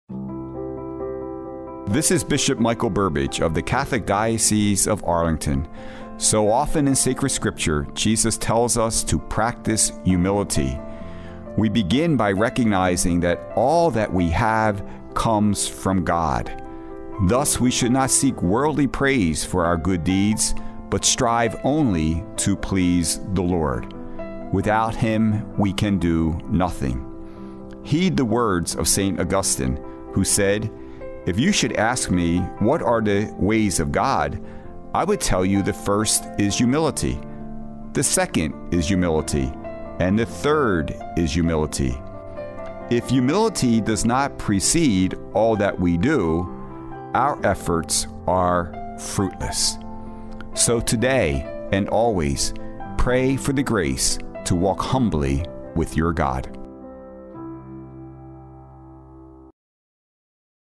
Humility - One Minute with Bishop Burbidge
Jesus tells us to avoid all forms of hypocrisy. We begin by recognizing all that we have comes from God. Humility also helps as we must ensure that we do the right things for the right reasons, whether in daylight or darkness, in the company of others or alone. Thus we should not seek worldly praise for our good deeds but strive only to please God. Heed the words of St. Augustine: If you should ask me what are the ways of God, I would tell you the first is humility; the second is humility; and the third is humility. If humility does not precede all that we do, our own efforts are fruitless. So today and always, walk humbly with your God.